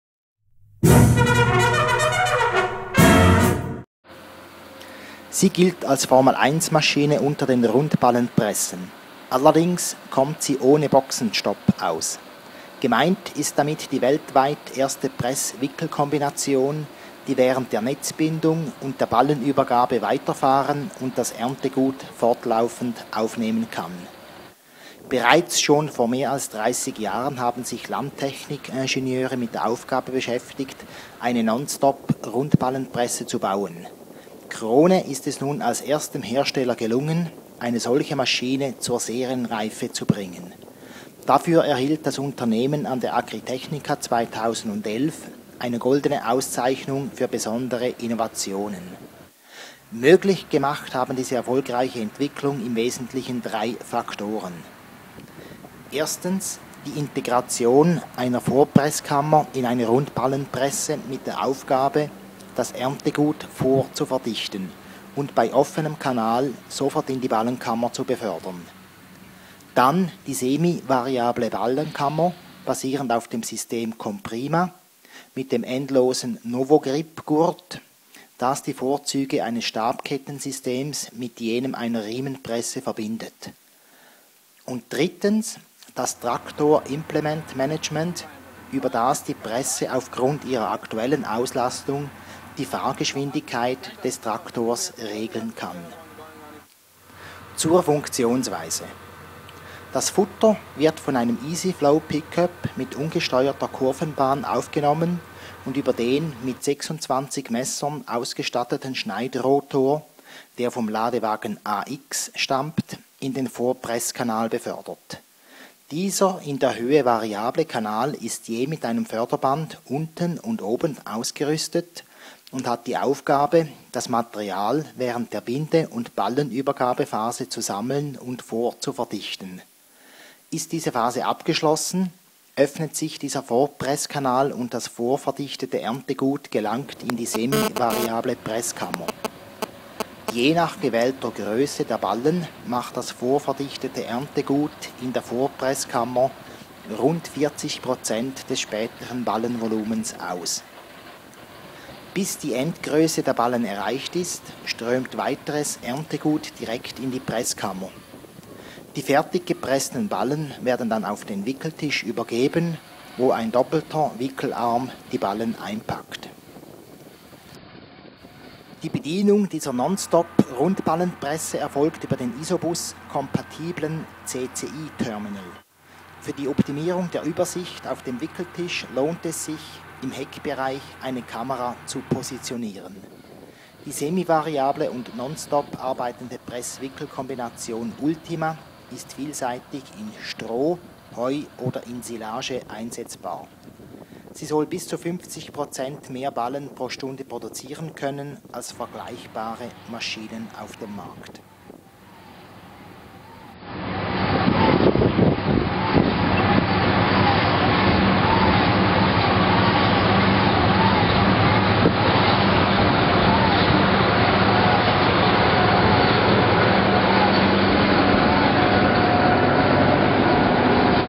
Press-Wickel-Kombination «Ultima» von Krone
Krone stellte die an der Agritechnica mit Gold prämierte, weltweit erste kontinuierlich arbeitende Press-Wickel-Kombination «Ultima» in der Nähe von Salzbergen (D) erstmals in einem praktischen Einsatz der Öffentlichkeit vor (Aufnahmen: Roman Engeler).